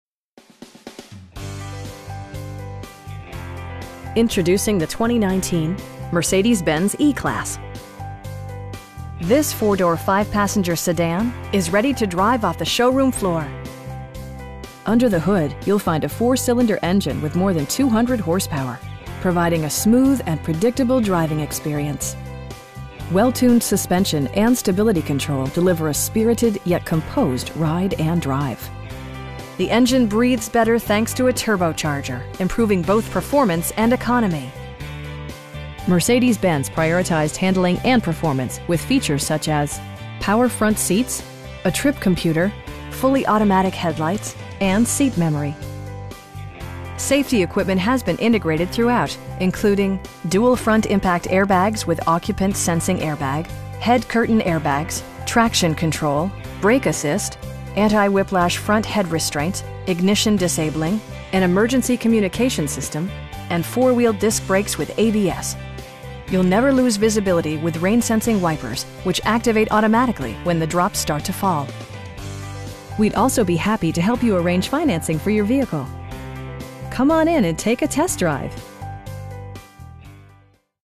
2019 Mercedes-Benz E-Class E 300 4MATIC
Mercedes-Benz of New London

Introducing the 2019 Mercedes-Benz E-Class. This 4 door, 5 passenger sedan is ready to drive off the showroom floor! Under the hood you'll find a 4 cylinder engine with more than 200 horsepower, providing a smooth and predictable driving experience. Well tuned suspension and stability control deliver a spirited, yet composed, ride and drive The engine breathes better thanks to a turbocharger, improving both performance and economy. Mercedes-Benz prioritized handling and performance with features such as: a tachometer, power front seats, a trip computer, fully automatic headlights, cruise control, an overhead console, and seat memory. Safety equipment has been integrated throughout, including: dual front impact airbags with occupant sensing airbag, head curtain airbags, traction control, brake assist, anti-whiplash front head restraints, ignition disabling, an emergency communication system, and 4 wheel disc brakes with ABS. You'll never lose visibility with rain sensing wipers, which activate automatically when the drops start to fall. We'd also be happy to help you arrange financing for your vehicle. Come on in and take a test drive!